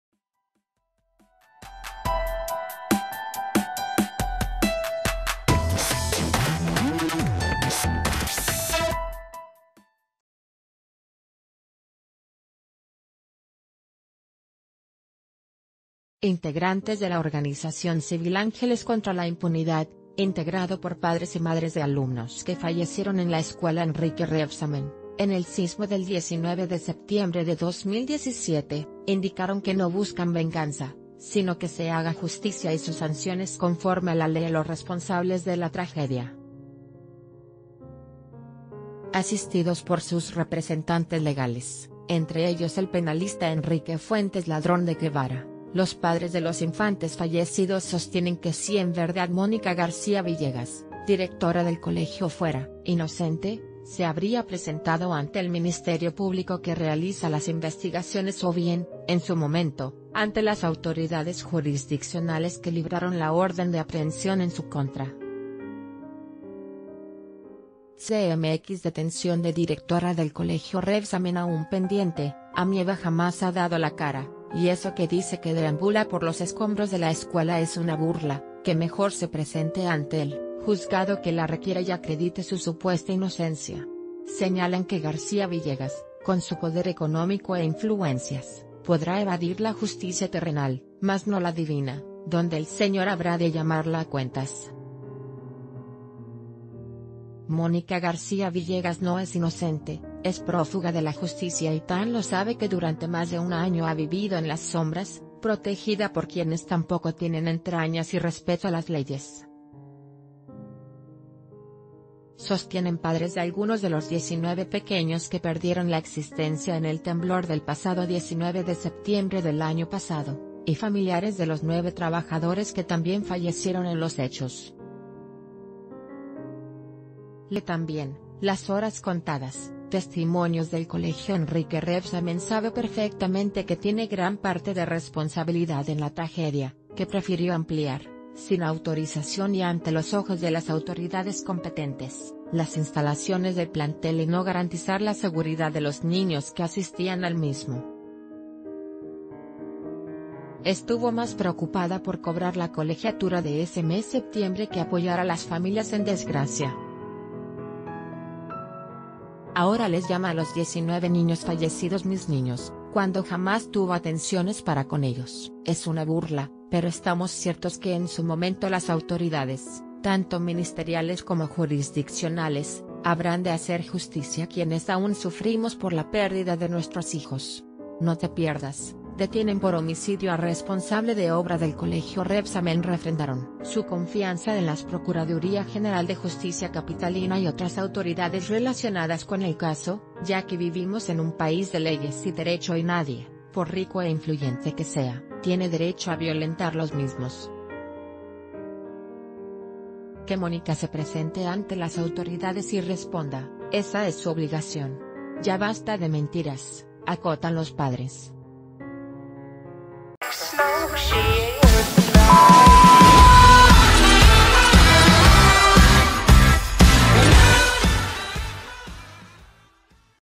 Directora del Rébsamen no es inocente: padres de niños fallecidos exigen que dé la cara
Integrantes de la organización civil “Ángeles contra la Impunidad”, integrado por padres y madres de alumnos que fallecieron en la escuela Enrique Rébsamen, en el sismo del 19 de septiembre de 2017, indicaron que no buscan venganza, sino que se haga justicia y se sanciones conforme a la ley a los re...